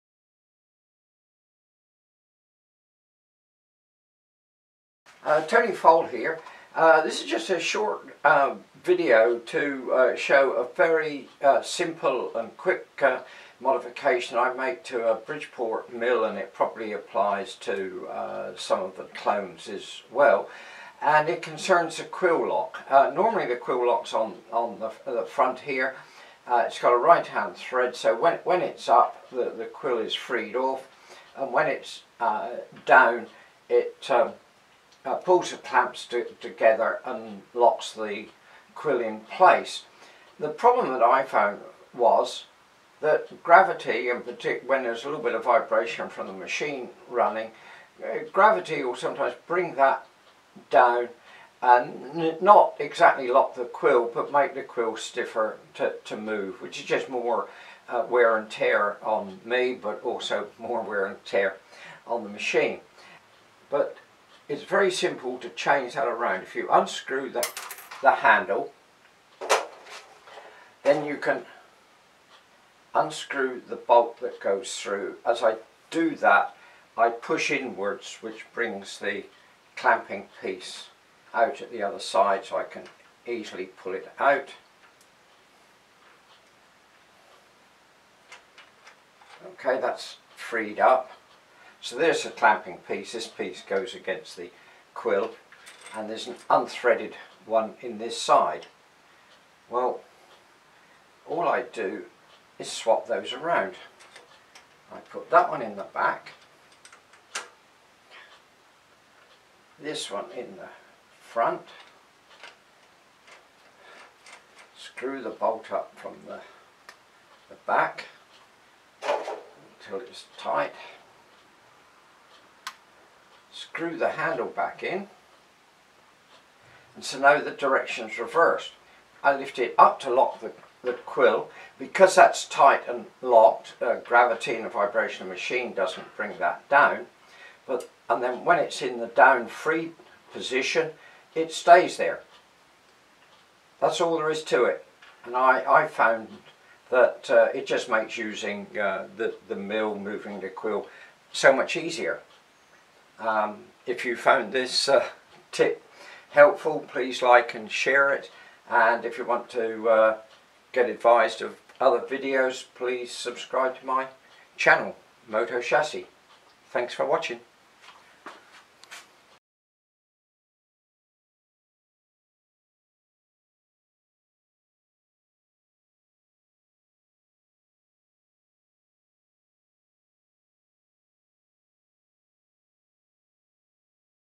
Milling machine Quill Lock improvement.
A 3 minute video showing a quick and simple but useful quill lock improvement for a milling machine.
The modification requires no machining nor fabrication but improves the utility of the machine.